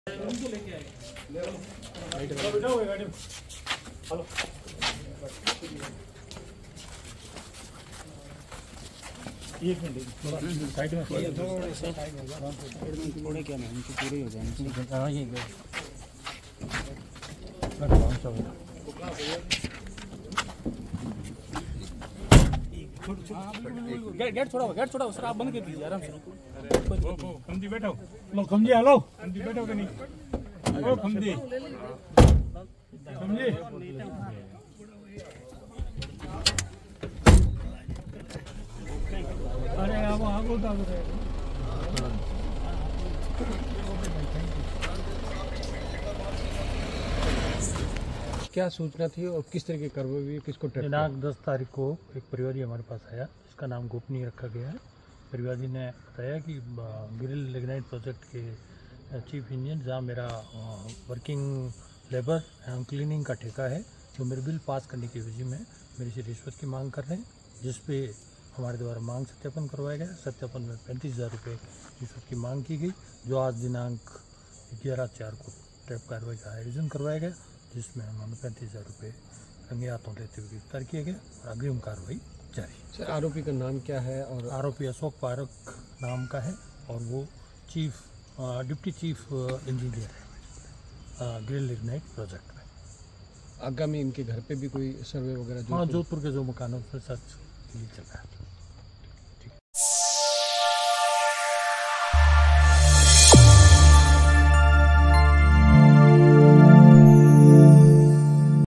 चीफ इंजीनियर को पुलिस ने रंगे हाथों रिश्वत लेते पकड़ा, पुलिस ने 35 हज़ार रुपए किए जब्त| Barmer News
About this Video:

चीफ इंजीनियर को पुलिस ने रंगे हाथों रिश्वत लेते पकड़ा, पुलिस ने 35 हज़ार रुपए किए जब्त| Barmer News | 12.April.2023